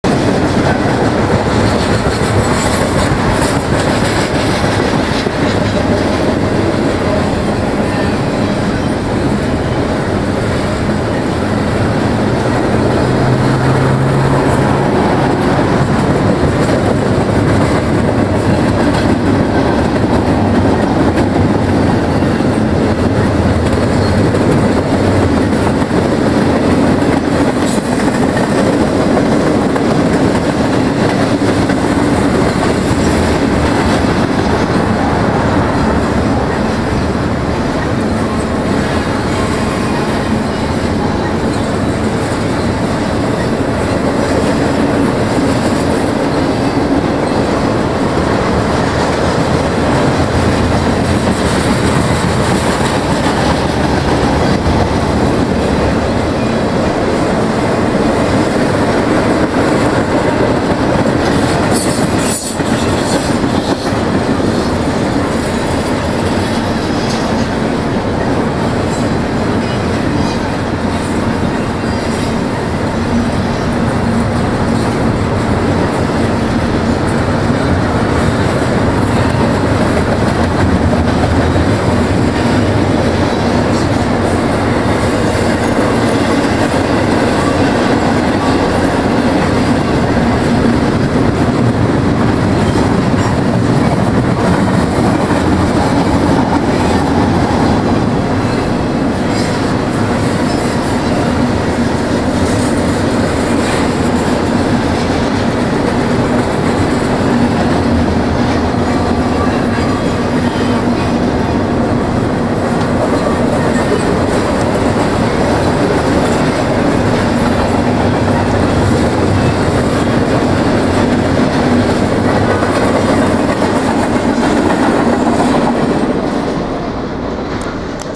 Norfork Southern Eastbound Intermodal Train I (HD) Douglasville,Ga. 6-3-2013
Norfork Southern Intermodal.Last Train Shot Of The Day.
NS ???
NS ???
NS ???
10:45AM Monday